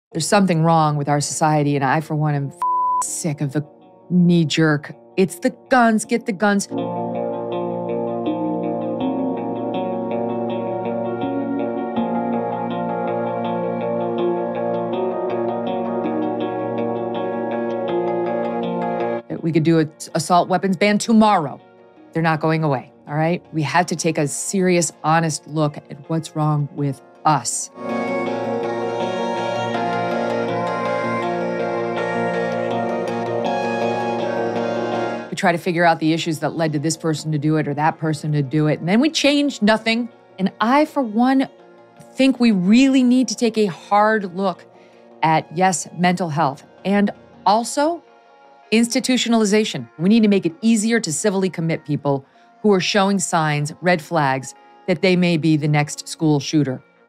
Megyn Kelly ‘f**king sick’ of calls to ban guns after Nashville shooting
American journalist Megyn Kelly slammed calls for gun control following the mass shooting at a Nashville school that left six people, including three children, dead.